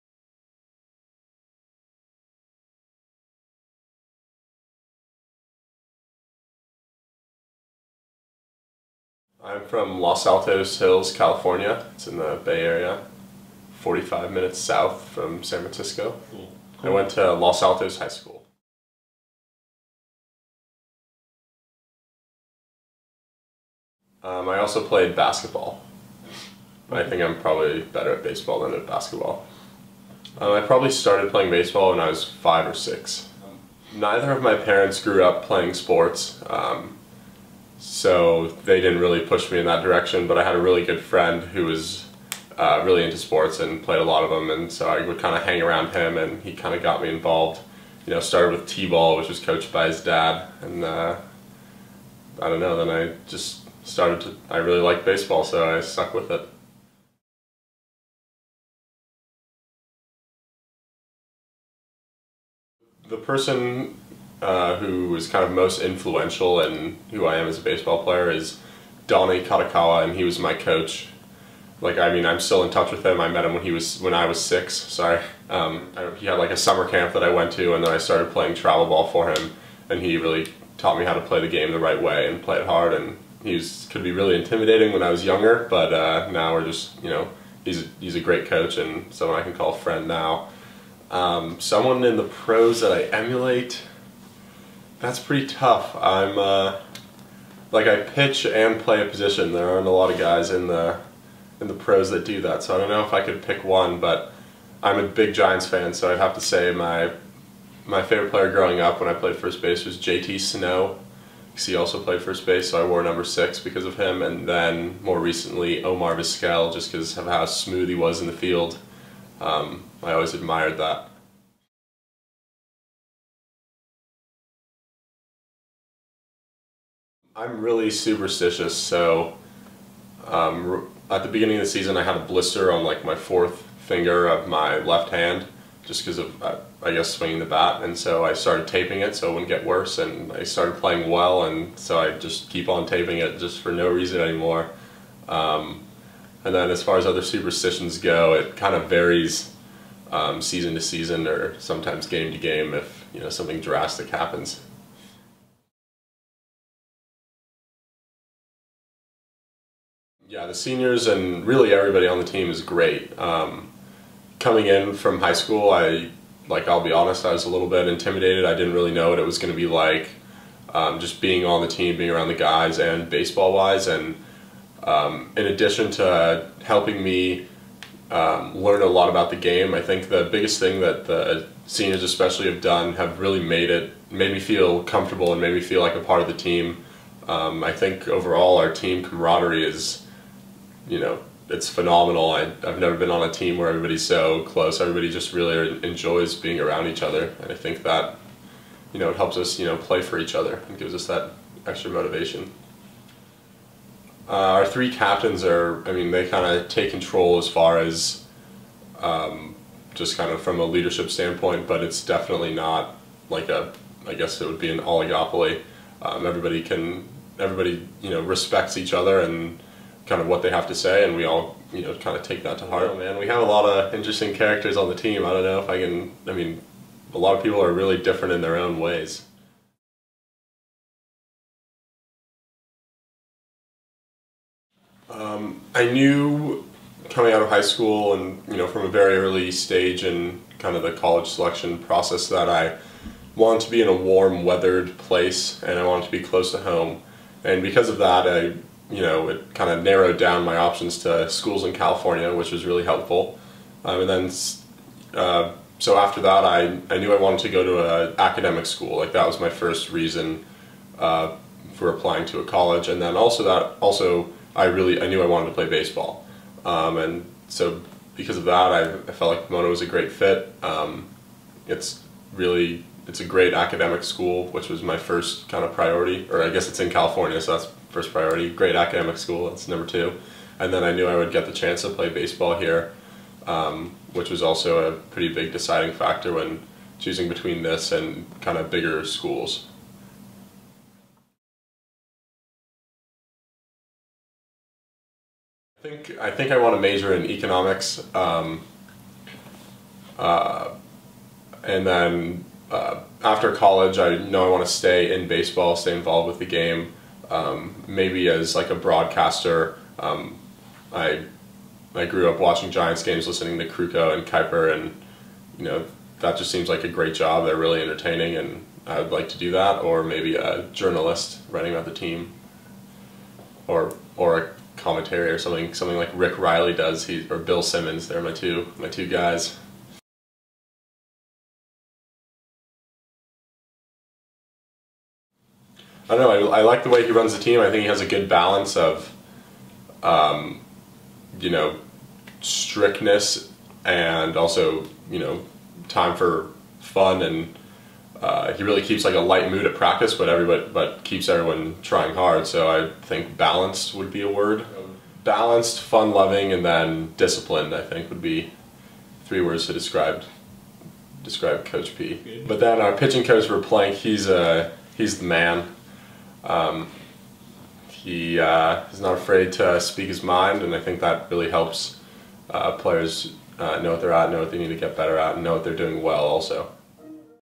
PP Baseball: Simon Rosenbaum
A conversation with Pomona-Pitzer's baseball player, Simon Rosenbaum.

Follow Pomona-Pitzer Athletics on Twitter: @sagehens